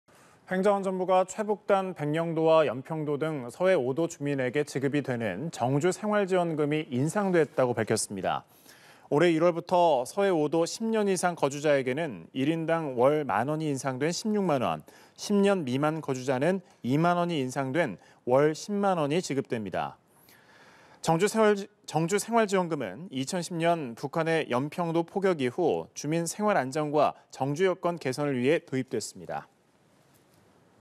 서해 5도 정주생활지원금 월 10만~16만원으로 인상 / 연합뉴스TV (YonhapnewsTV)
서해 5도 정주생활지원금 월 10만~16만원으로 인상

행정안전부는 최북단 백령도와 연평도 등 서해 5도 주민에게 지급되는 정주생활지원금이 인상됐다고 밝혔습니다.

올해 1월부터 서해 5도 10년 이상 거주자에게는 1인당 월 1만원 인상된 16만원, 10년 미만 거주자는 2만원 인상된 월 10만원이 지원됩니다.

정주생활지원금은 2010년 북한의 연평도 포격 이후 주민 생활안정과 정주여건 개선을 위해 도입됐습니다.